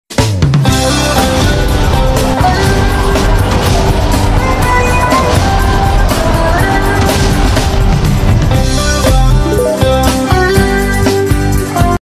BAG EXERCISE 3
BANG A BAG. Sweat is Sweet in Physical Fitness Programs. Work Out!

Shadowless Kick na parang latigo hagupit ng sipa.

Sa mga nakaabot sa pulang bag na iyan nung 2009 sa Phil Kamao Gym, alam nyong parang sako ng semento ang lower part na iyan.